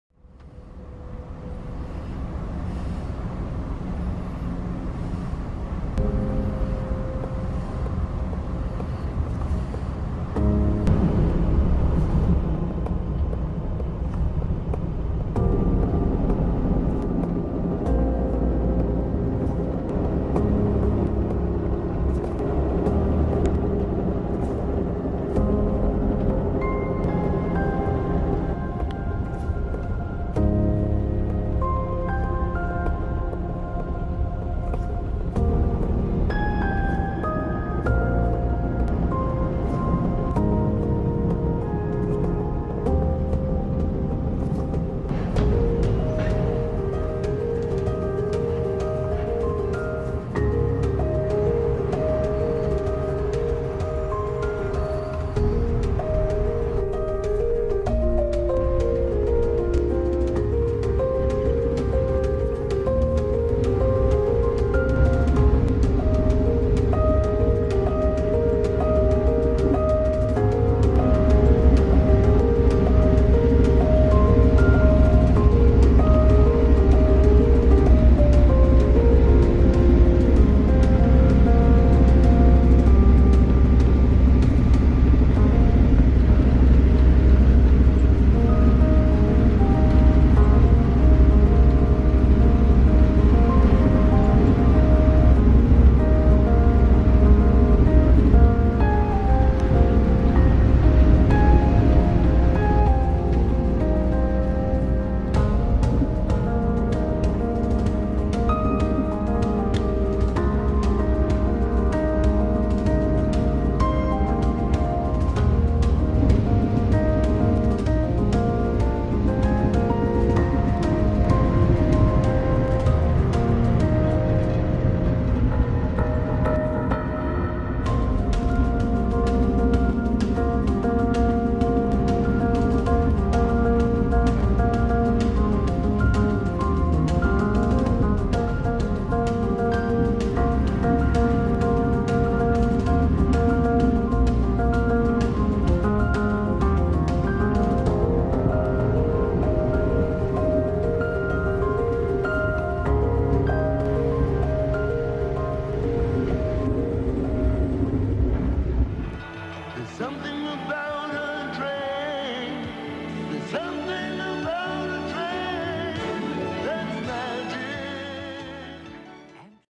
Jaw-dropping Amtrak Train Journeys: Chasing The Sun From Dawn To Dusk!
Silver Star Miami to NYC Sunrise & Empire Service Hudson Valley Sunset aboard Amtrak Inspiring rides